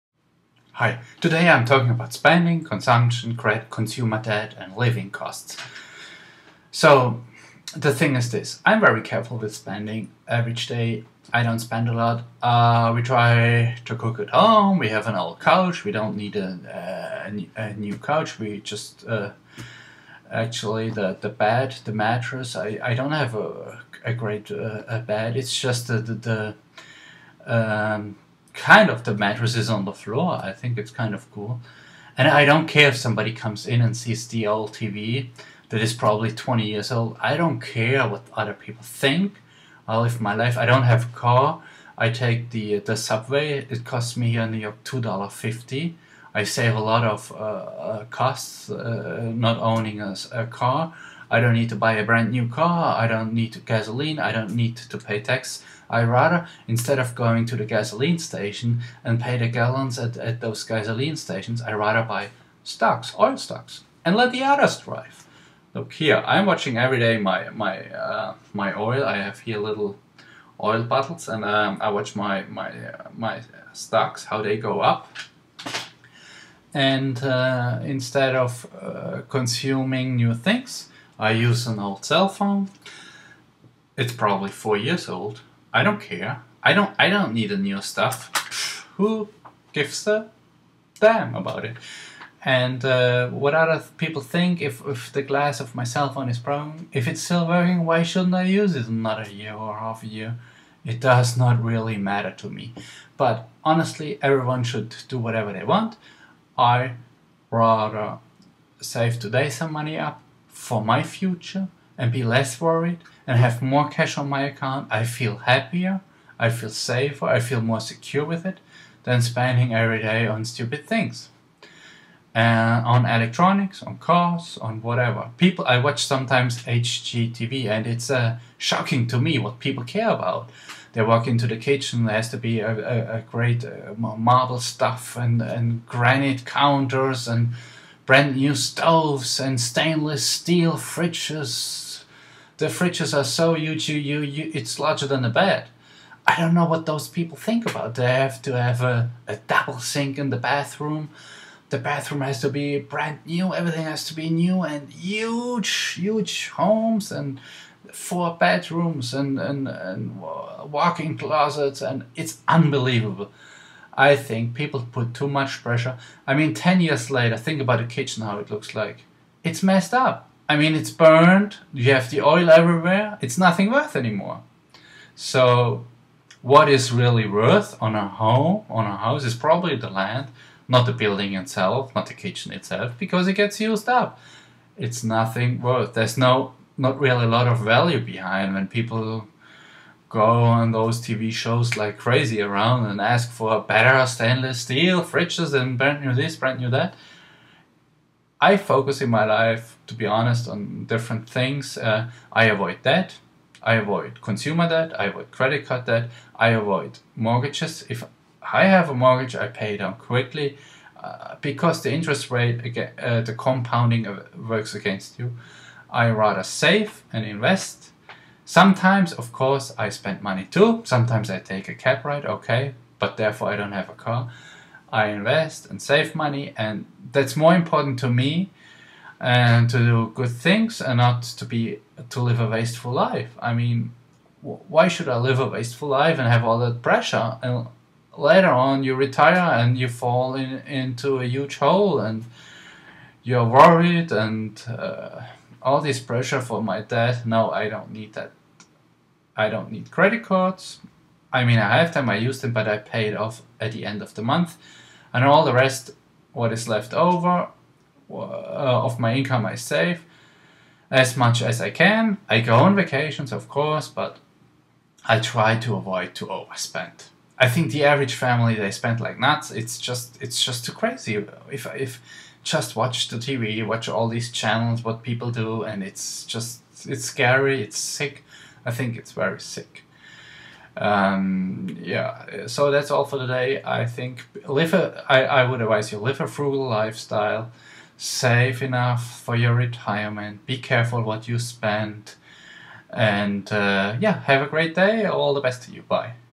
The Crazy Spending Spree of an Average Household
New car, new house, new cloth. The best stuff you can get. Of course. People like to finance everything. Mortgages, bank loans, credit card debt. Wonderful. They have to work till they are dead. They can never enjoy financial freedom.